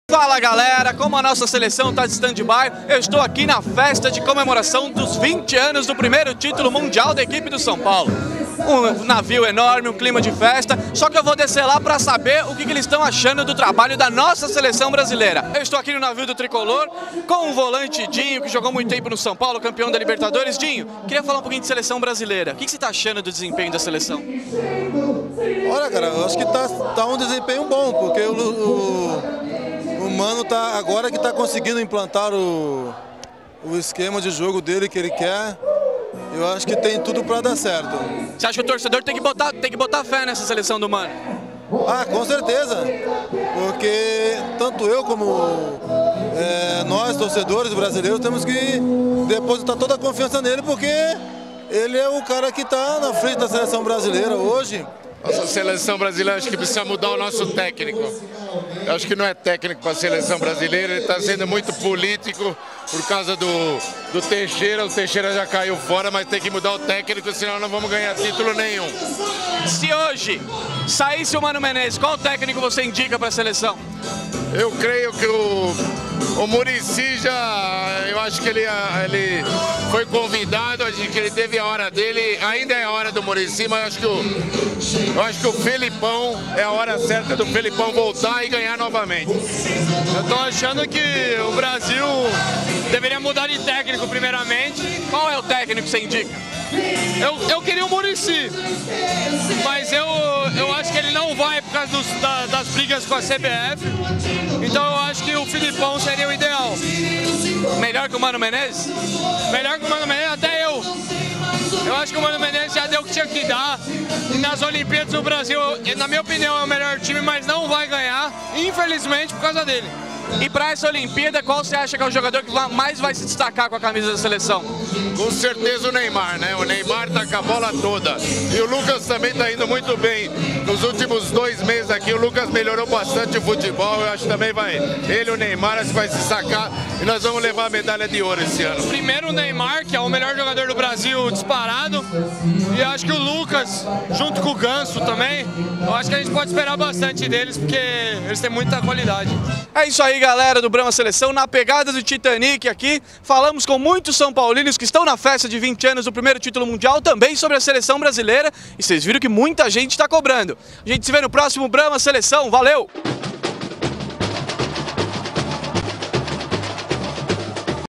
Mano Menezes é o técnico certo para Seleção Olímpica?
Daniel Gouveia conversa com torcedores são paulinos sobre Mano Menezes e Seleção Olímpica.